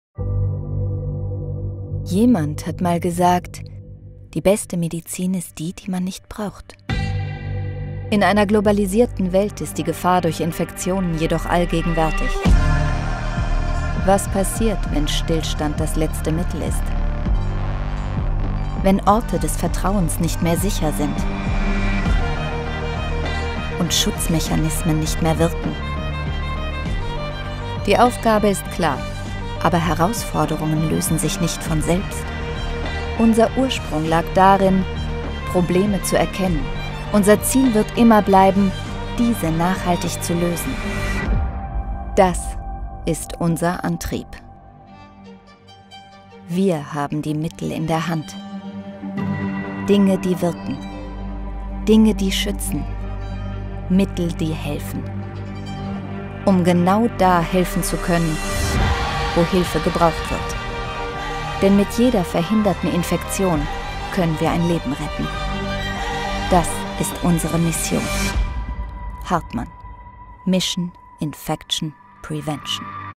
Mission: Infection Prevention - unsere neue globale Initiative zur Infektionsprävention
Nicht nur die jüngsten Ereignisse haben gezeigt, wie wichtig es ist, sich effektiv vor einer Infektion zu schützen - insbesondere in Krankenhäusern und professionellen Gesundheitseinrichtungen. Die Initiative „Mission: Infektionsprävention“ umfasst alle Möglichkeiten und Erfahrungen, die HARTMANN seinen Kunden auf dem Gebiet der Infektionsprävention anbieten kann.